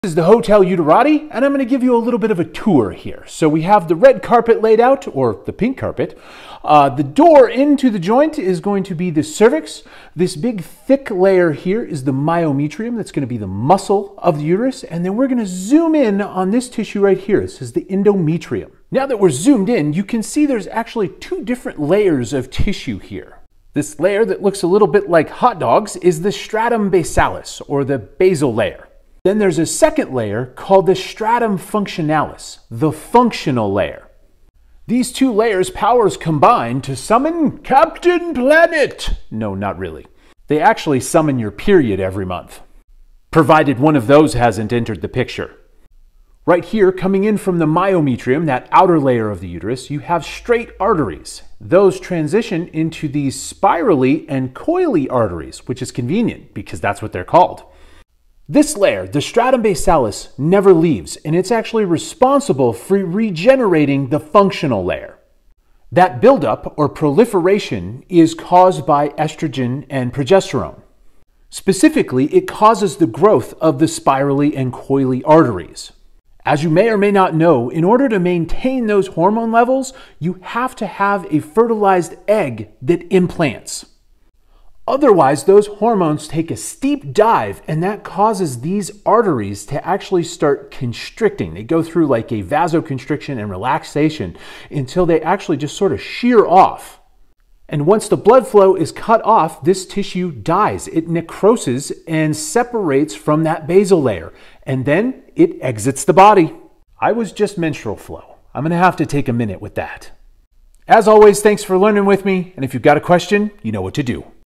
Let's Go Inside the Hotel Uterati
The Endometrium: Your Uterus's Welcome Mat

Imagine your uterus as a cozy little home for a potential baby. The endometrium is like the soft, cushiony lining inside that home, preparing itself every month for the possibility of hosting a tiny guest.

The endometrium is a crucial part of the female reproductive system. It's a specialized tissue lining the inside of the uterus. Its main job is to support and nourish a fertilized egg, allowing it to grow into a baby if pregnancy occurs.

Monthly Dance:

Every month during a woman's menstrual cycle, the endometrium gets ready for the possibility of pregnancy. When an egg is released from the ovaries (ovulation), hormones in your body signal the endometrium to thicken and become rich in blood vessels. This lush environment provides the perfect setting for a fertilized egg to implant and start growing.

If No Baby Arrives:

If fertilization doesn't occur, the levels of certain hormones drop, signaling the endometrium that it's time to start anew. This leads to the shedding of the endometrial lining during your period, and the cycle starts all over again.

Pregnancy and the Endometrium:

If an egg is successfully fertilized and implants itself into the endometrium, the lining becomes even more important. It will continue to support and nourish the developing embryo throughout the pregnancy until the placenta takes over this role.

A Dynamic Tissue:
The endometrium is no passive lining; it's a dynamic tissue that undergoes changes in response to hormones, preparing itself for pregnancy each month. If pregnancy occurs, it adapts to support the growing baby; if not, it resets itself for the next cycle.

Importance of the Endometrium:
Having a healthy endometrium is vital for a woman's reproductive health. A receptive and well-nourished endometrial lining increases the chances of successful pregnancy. Doctors may assess the thickness and quality of the endometrium during fertility evaluations and other reproductive health check-ups.

So, the endometrium plays a crucial role in the beautiful journey of creating new life and serves as a nurturing nest for a potential baby. It's an incredible example of how our bodies prepare for the possibility of life with each passing month